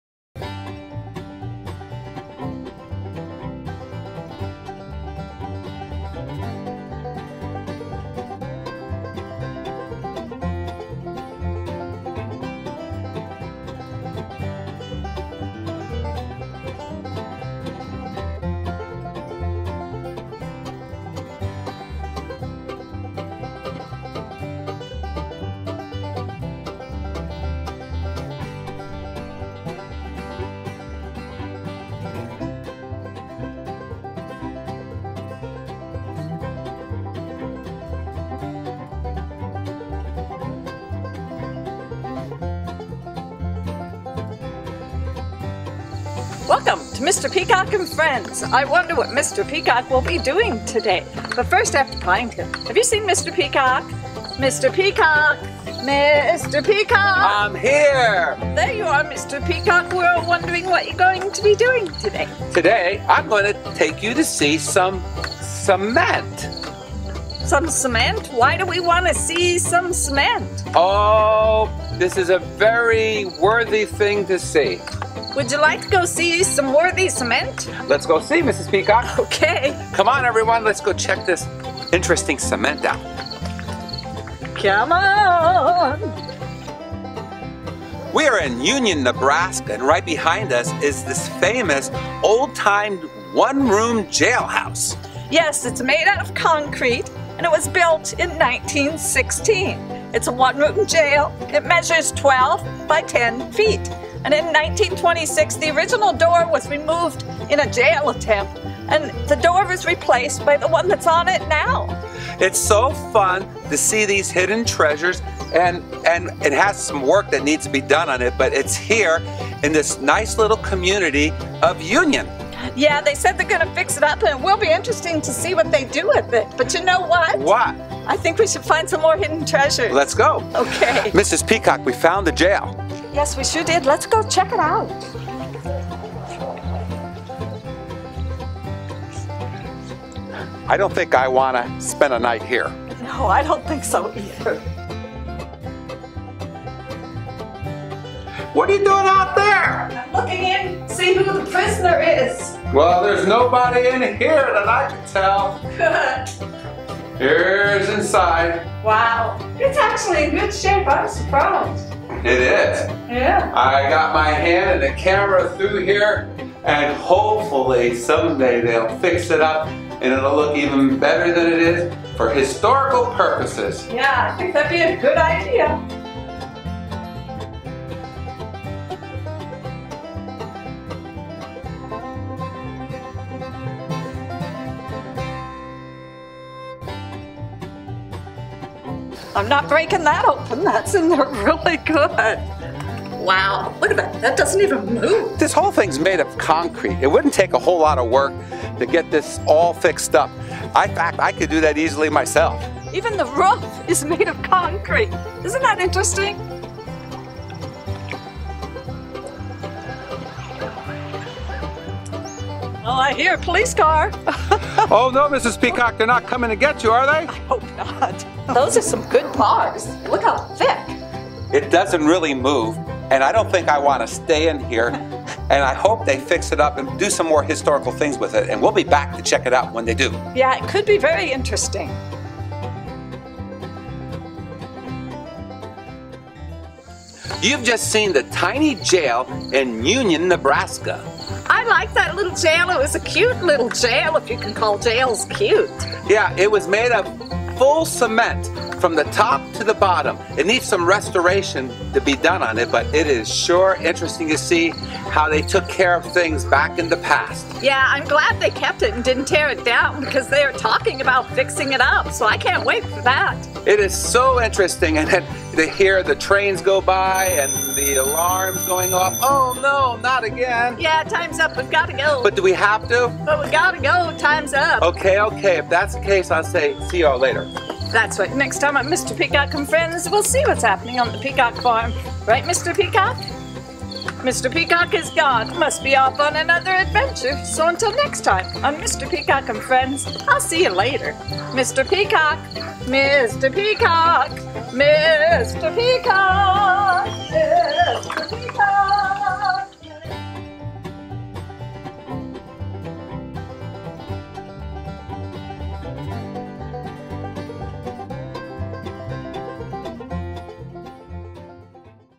Tiny Cement Jail, Union, Ne. Travel USA, Mr. Peacock & Friends, Hidden Treasures
On Mr. Peacock’s & Mrs. Peacock latest adventure, they discovered a hidden treasure of a Tiny Cement Jail in Union Nebraska. See this Tiny Cement JaIl, measuring 12 X 10 feet built in 1916 and on the register of historic places from Mr. Peacock & Mrs. Peacock in their latest adventure.

Please join us for every episode of Mr. Peacock & Friends hidden treasures by visiting Mr. Peacock & Friends